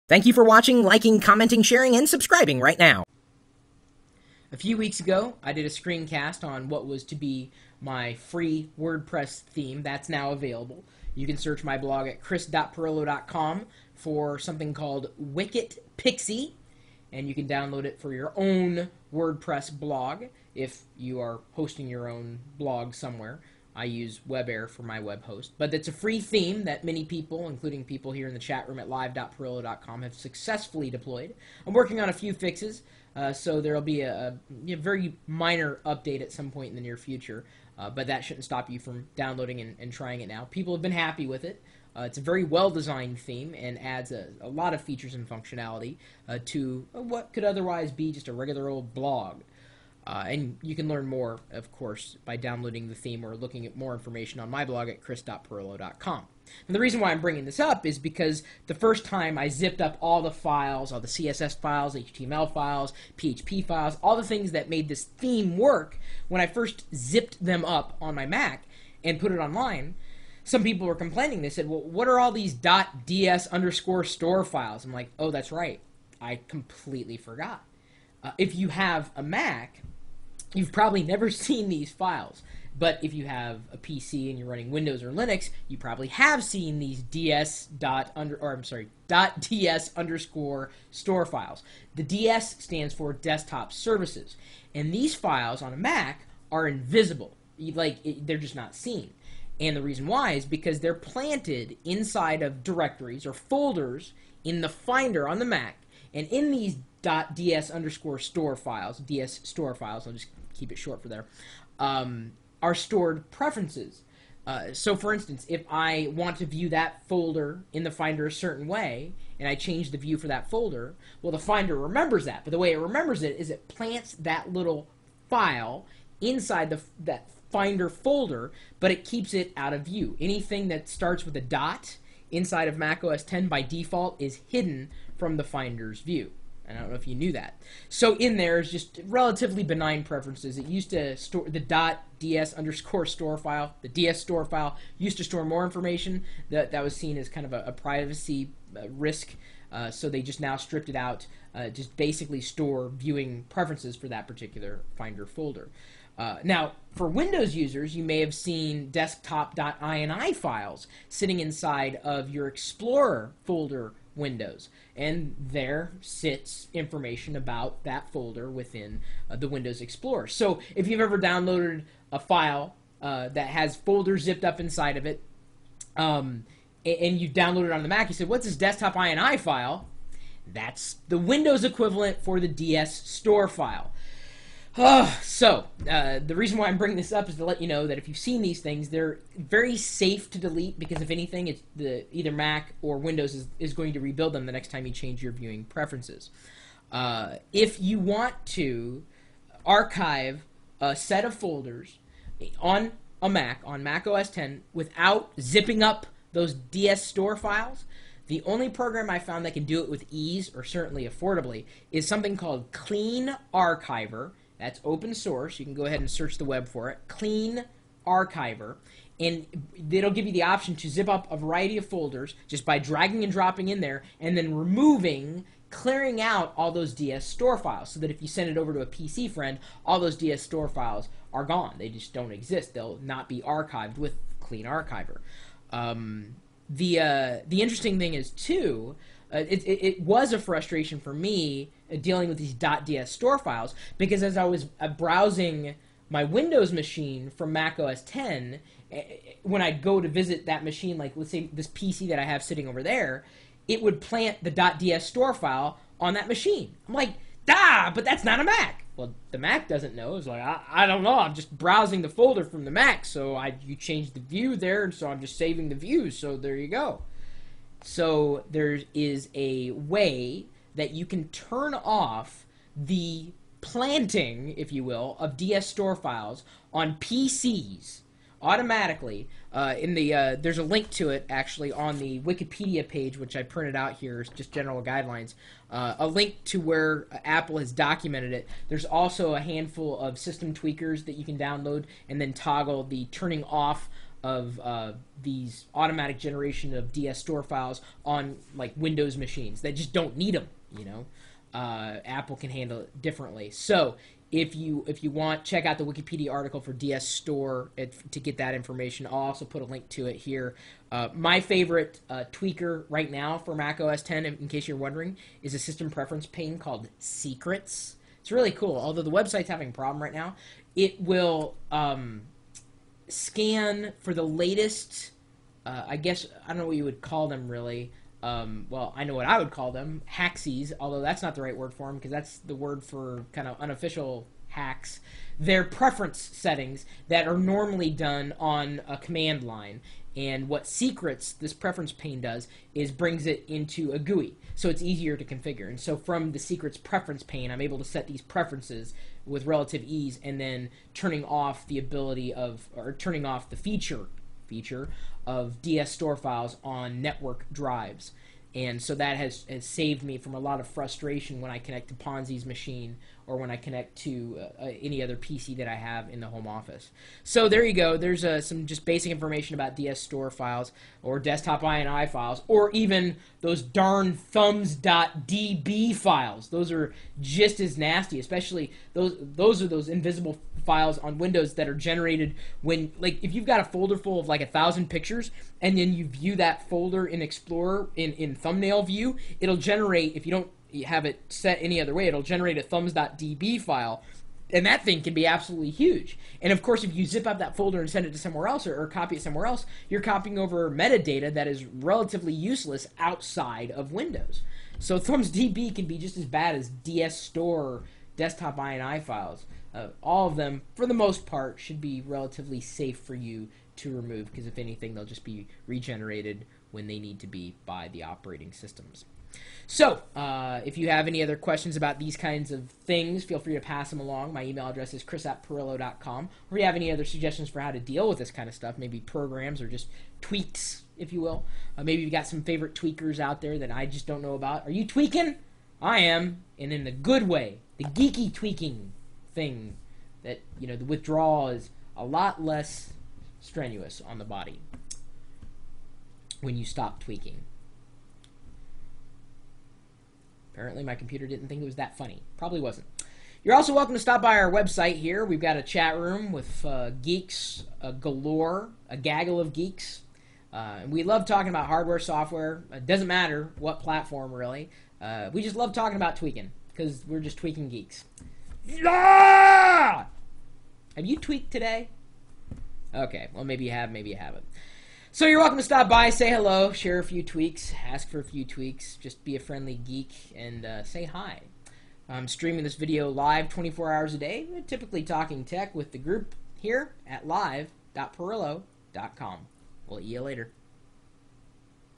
What are .DS_Store Files?
- A few weeks ago, I did a Screencast about my free WordPress theme, WicketPixie. The reason why I'm bringing this up, is that the first time I zipped all the files up on my Mac to put online, people were complaining. They didn't know what the .DS_Store files were. Blog post /2008/06/15/what-are-ds_store-files/